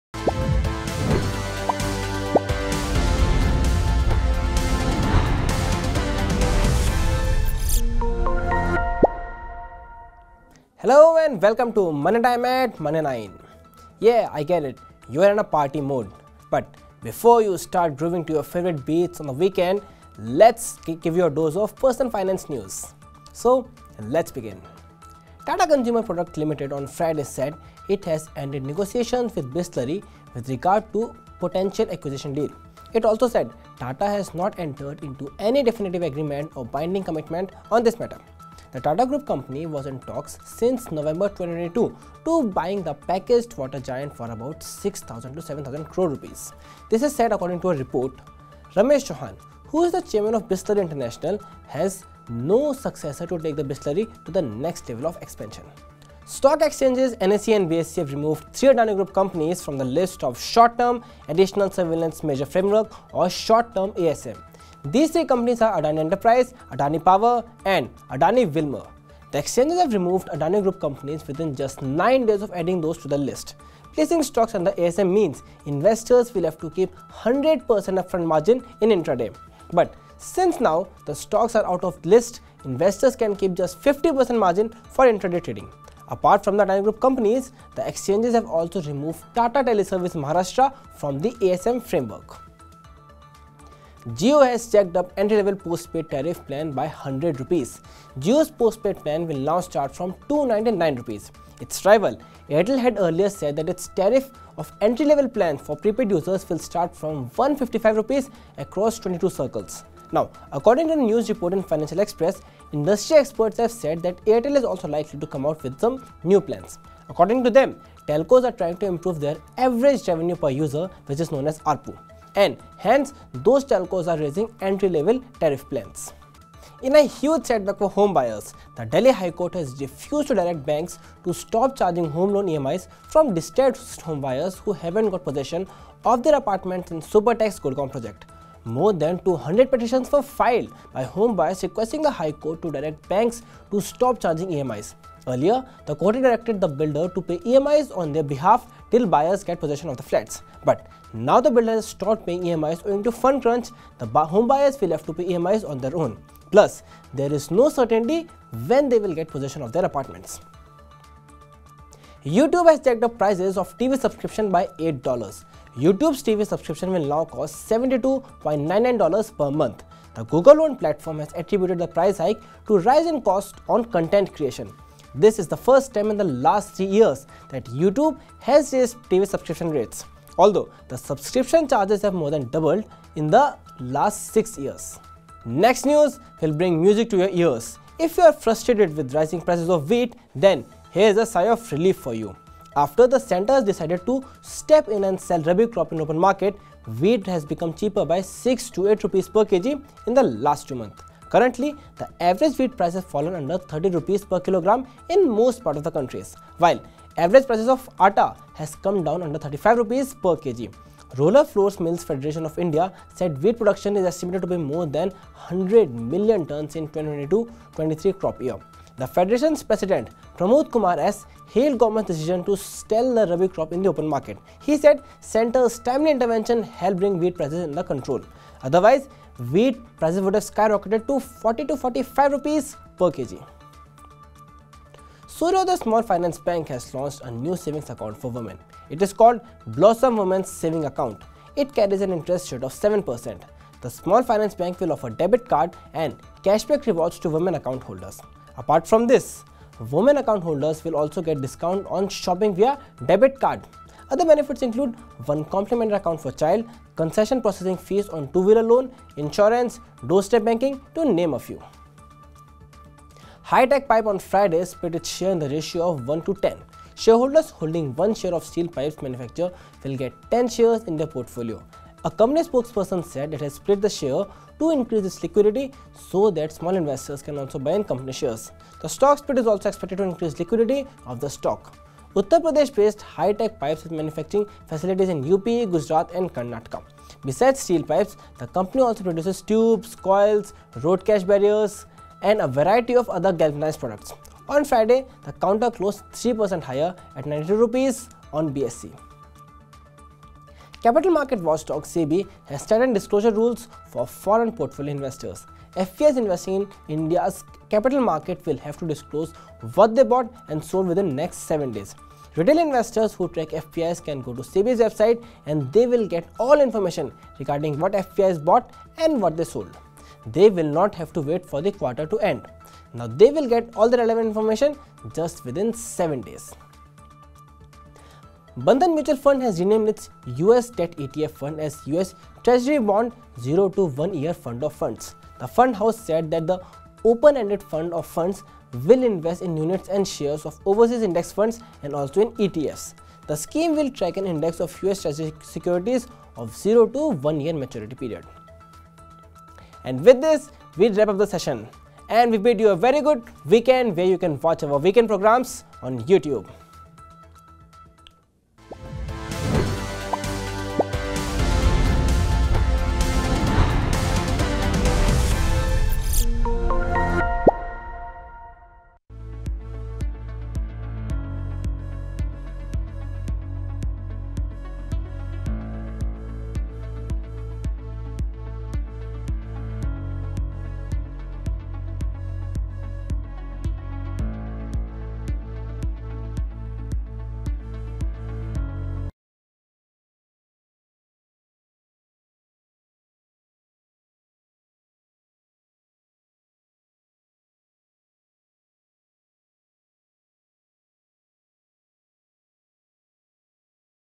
Tata ends talks with Bisleri | Money Time | Money9 English
NSE, BSE  remove 3 Adani Cos short term ASM; Jio's base postpaid plan costlier by Rs 100; YouTube hikes TV subscription fees